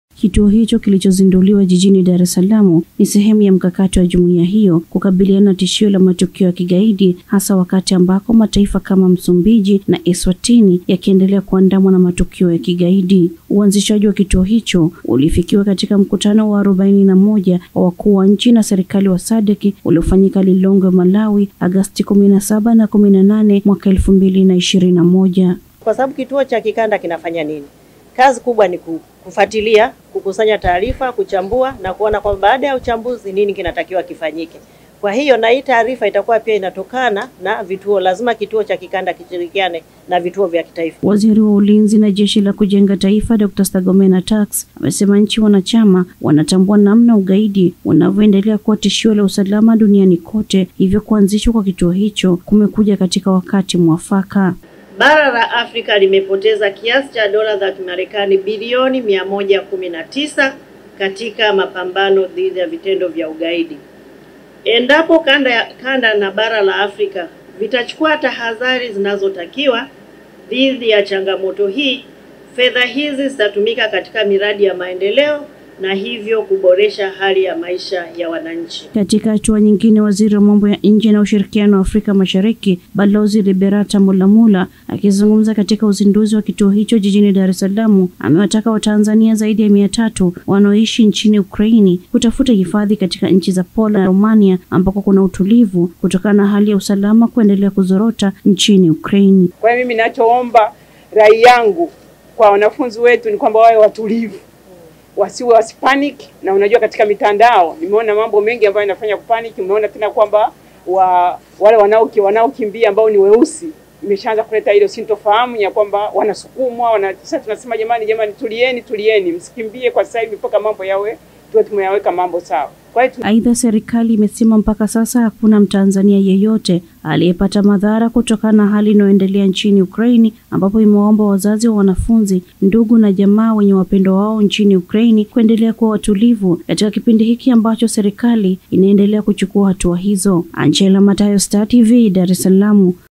UGAIDI SASA BASI, SADC YAZINDUA KITUO CHA KUPAMBANA NA AINA ZOTE ZA UGAIDI.
Nchi wanachama wa Jumuiya ya Maendeleo Kusini mwa Afrika SADC zimezindua kituo kitakachosaidia kuimarisha uwezo wa kupambana na ugaidi badala ya kusubiri mpaka nchi wanachama zishambuliwe ili kujua kinachoendelea na kudhibiti.